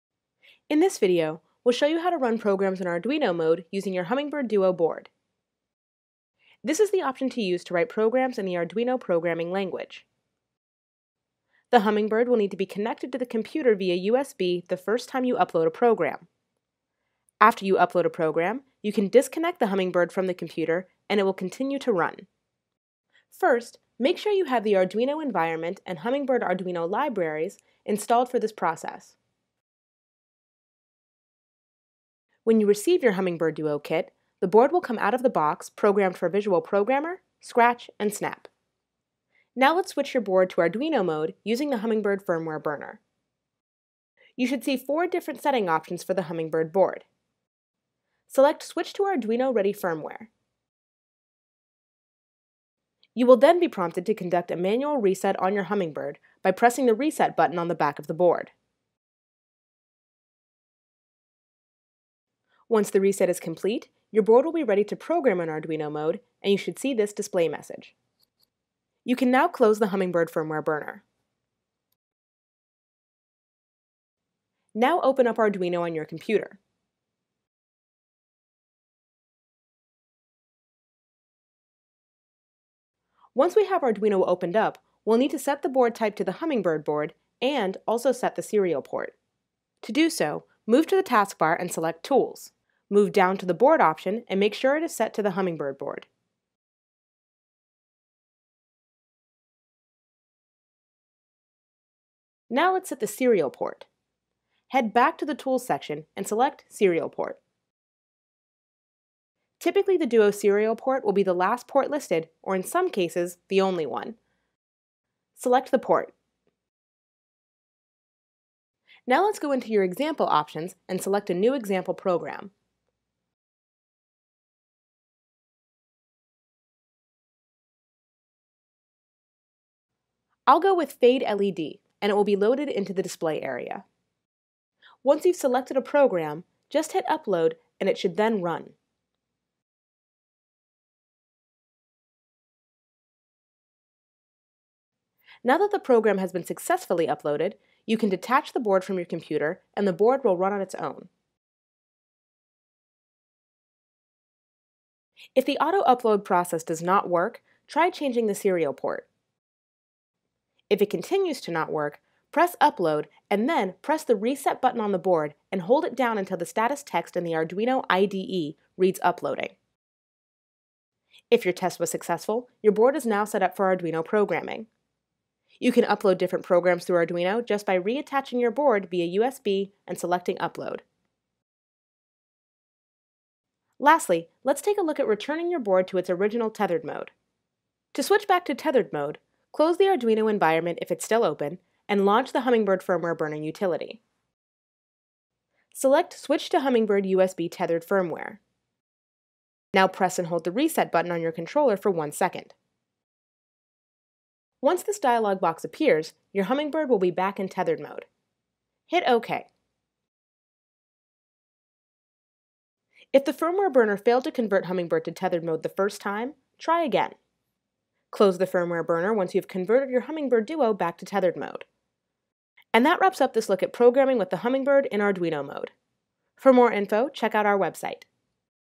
Arduino Programming
Let's take a look at setting up and programming with Arduino using the Hummingbird!